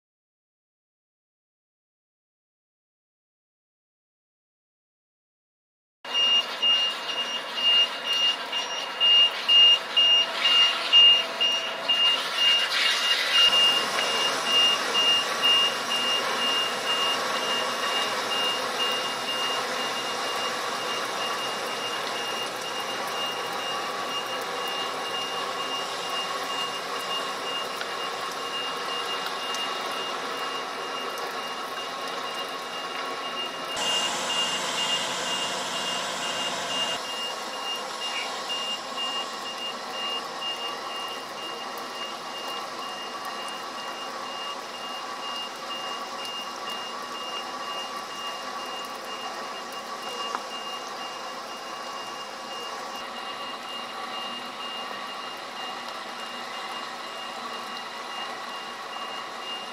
DIGITAL Studio Havířov - Záplavy 2010 - povodeň
Vyproštění vozu s přívěsem v Havířově Šumbarku.